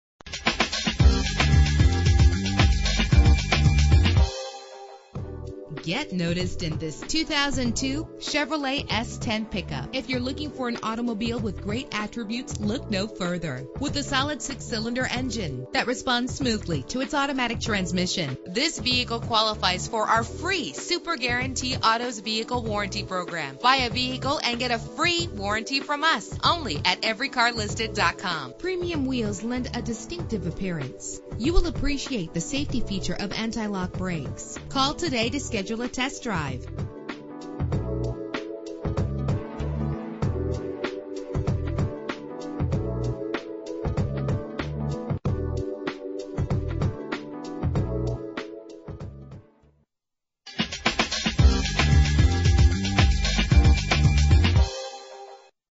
2002 Chevrolet S-10 Pickup - Penn Laird VA
Year: 2002
Make: Chevrolet
Model: S-10 Pickup
Trim: LS ZR2
Engine: 4.3 liter 6 cylinder 12 valve
Transmission: Automatic
Color: Yellow
Mileage: 83410
Address:
6158 Spotswood Trail
Penn Laird, VA 22846